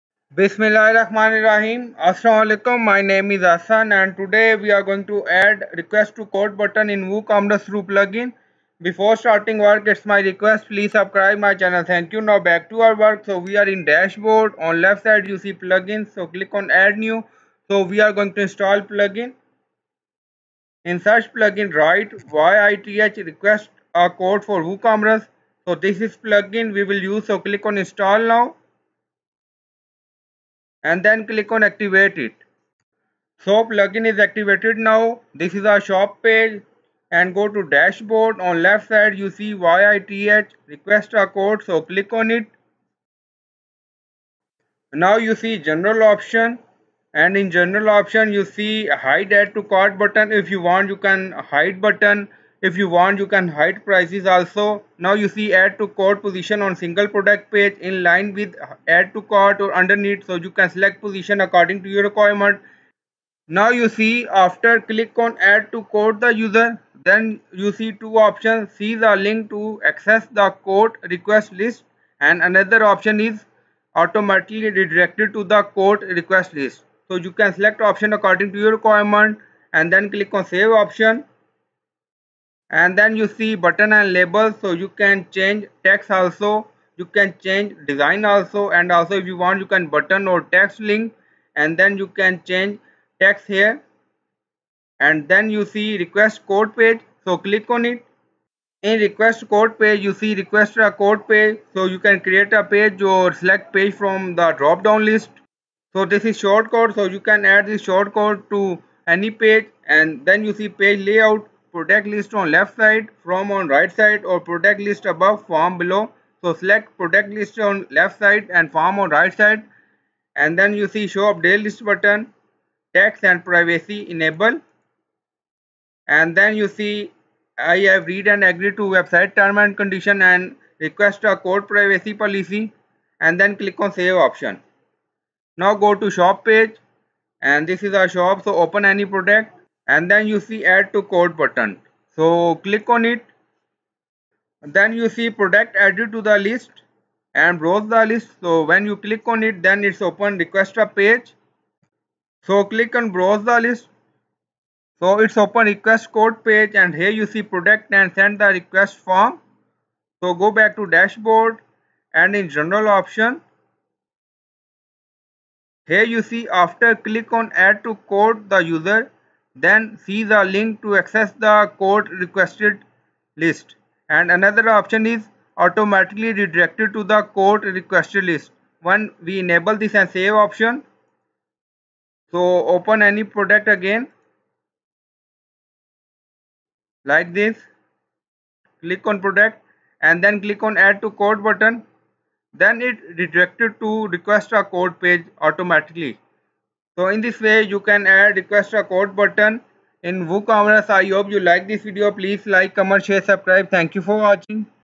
How to Add Request a Quote Button in Woocommerce | Product Enquiry Form Woocommerce WordPress
In this wordpress tutorial for beginners you will learn how to put request a quote button on products on shop page and when person open product then Add to Quote button appear and click on button to add product in the request quote page list and then user fill form for product enquiry through YITH Request a Quote for WooCommerce plugin in website.